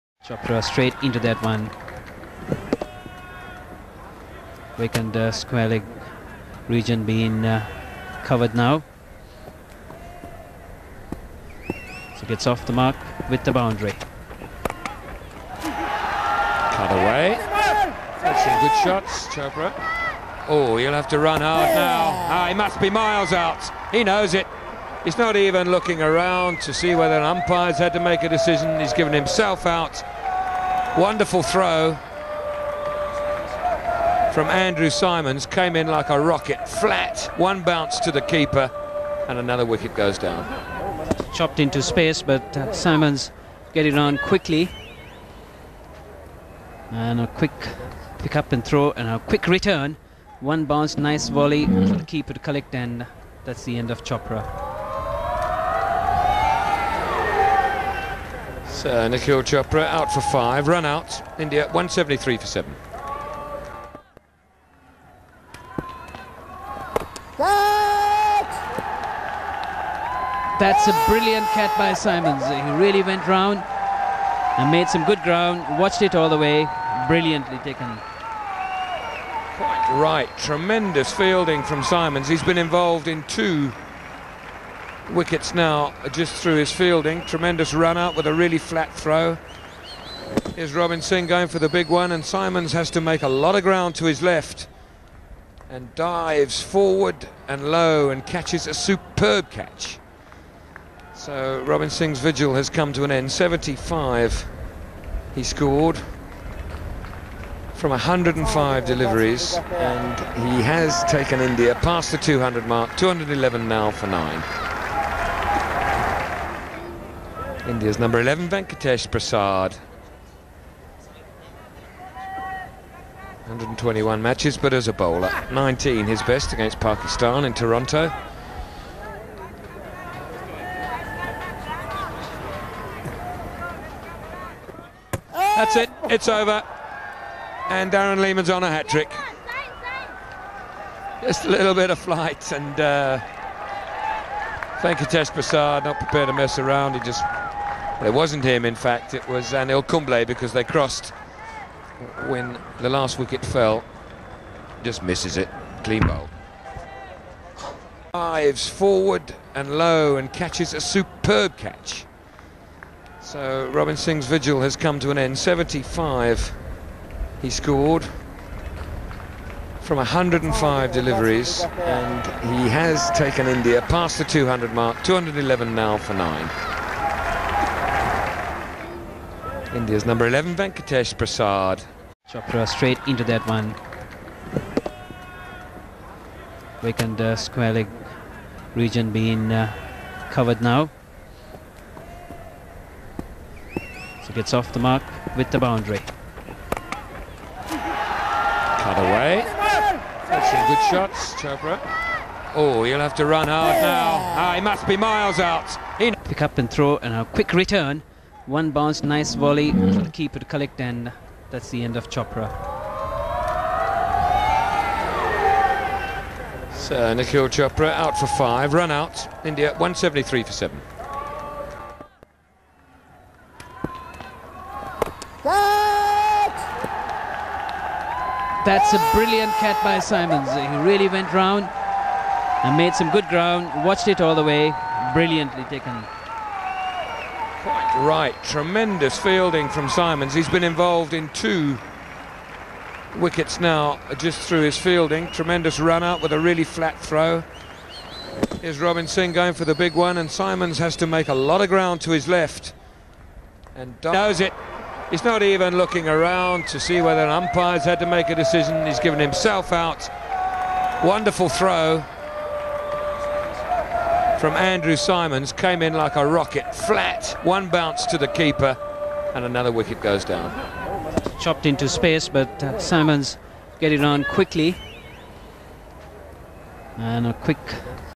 Great Fielding From Andrew Symonds
He's an incredible fielder. Full scorecard:

Andrew 'Roy' Symonds Fantastic All-Rounder.

James Brayshaw said his mum and dad will be asking for the DVD at the end of this video. Well, if they don't have it, or lost it since, they've got it now.